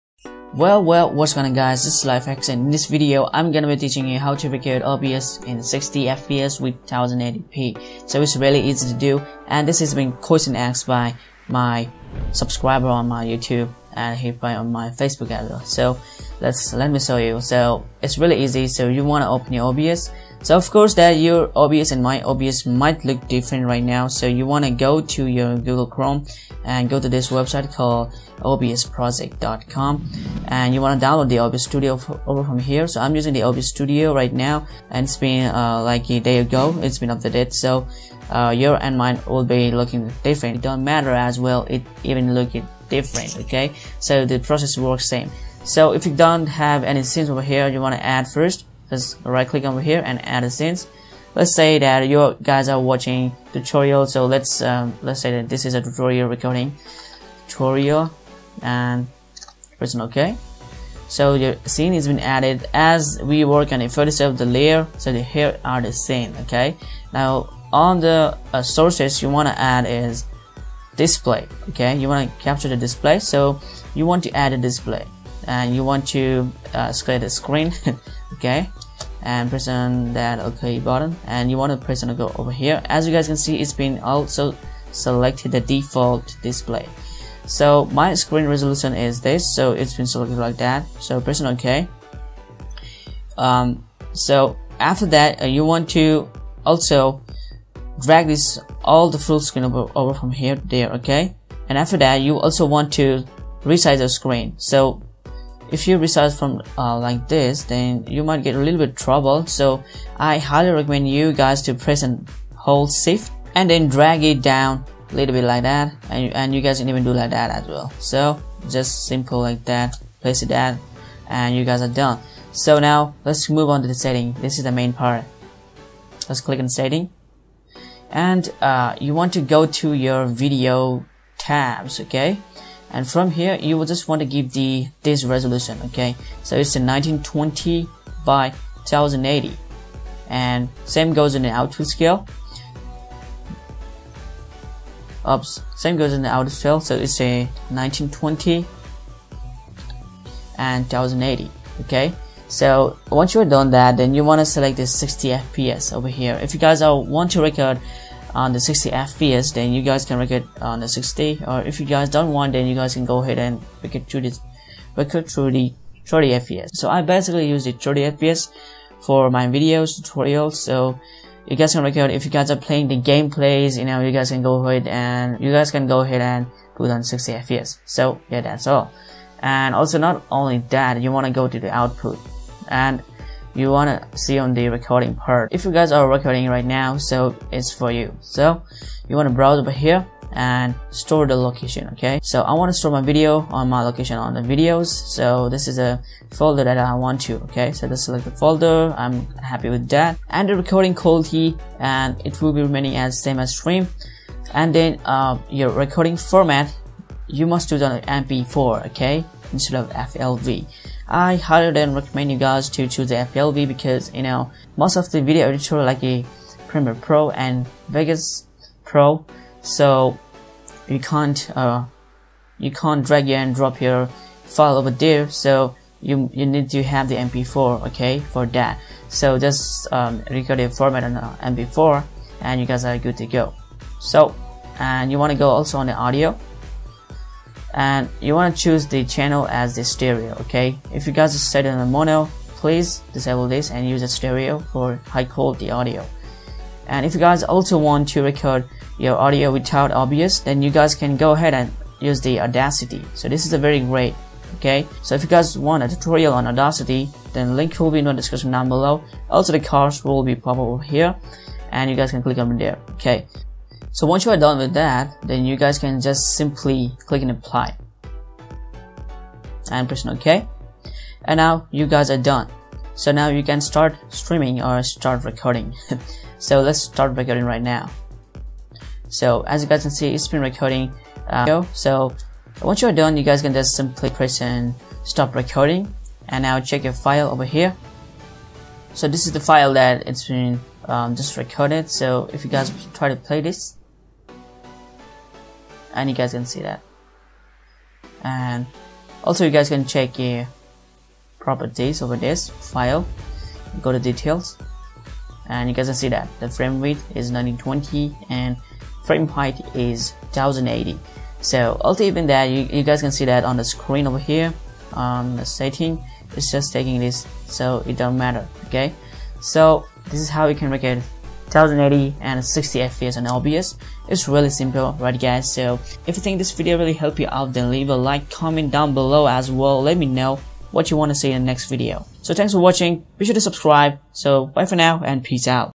How To Record Your Computer Screen For Free 2017 - Record 1080p 60fps with obs
Check out How To Record Your Computer Screen For Free 2017.
You can Record 1080p 60fps with obs! Obs is recording software and it is free and opensource too :)

Go ahead and record your computer screen for free!!  iT's damn easy as well :) !!!
Just follow up my tutorial your can easily fix it  :)!

⚠Thank you guys for watching my video hope you did enjoy!

♫Intro:
♫Outro:

~TheLifeHax